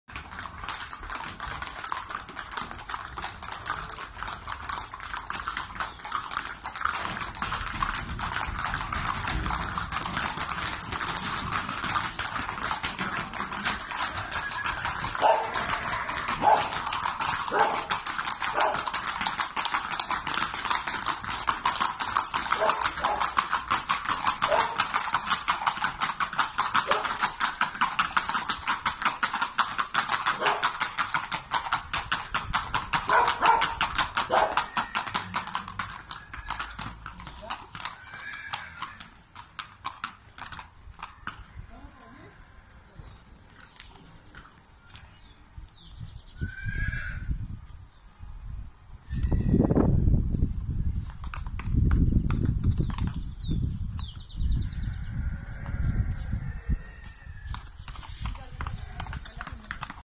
Embarque de la 1era tanda de yeguas para Agroexpointernacional 2019
En la tarde de ayer se embarcó la primera tanda de ejemplares para agroexpo @asdepaso.
Se espera hacer una buena presentación con:

MAESTRANZA Potranca Revelacion del P1
ILUCION FC , Multicampeona y reproductora del P1
MAGNIFICA DE LAS GUACAS FC Campeona Mundial y prototipo del P1
MENSAJERA "Supersonica" Campeona Mundial P3
MARAVILLA DE LAS MERCEDES FC Campeona Mundial y Madre de Campeones y Fueras de Concurso
TRADICION Linaje de Campeones mundiales Bucefafo y Euforia